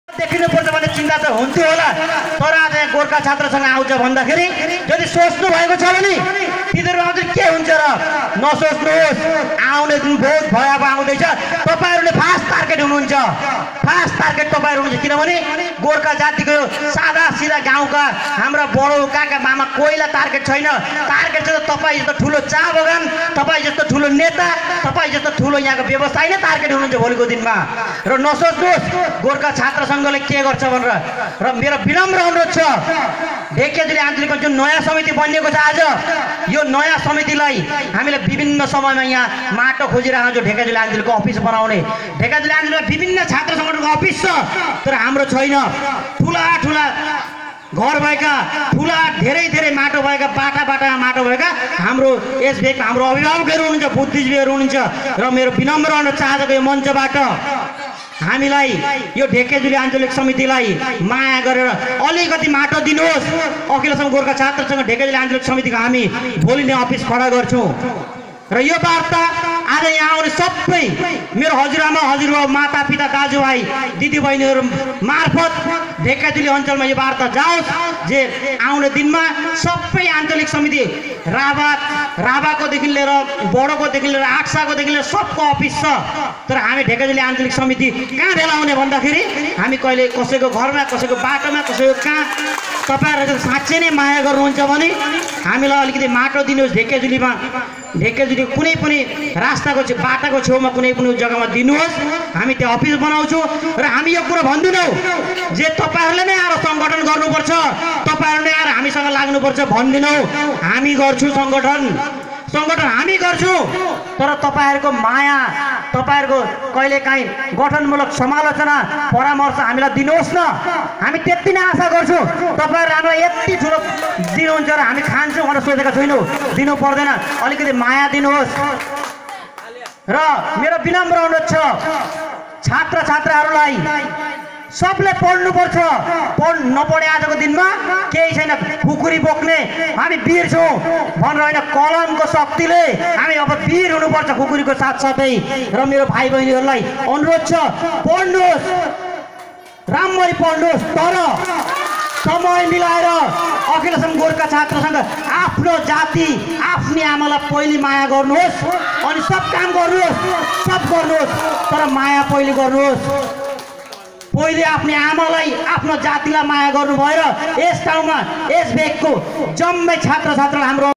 शोणितपुर जिला सभापति पुष्प पौडेलज्यु
हाम्रो खुकुरी मात्र धारभयेर हुदैन ,कलम पनि धारिलो हुनुपर्छ ,अब गोर्खालो सल्लाह खरानीको डल्ला हैन ढुङ्गाको डल्ला भैदेखाउनु पर्छ -पुष्प पौडेल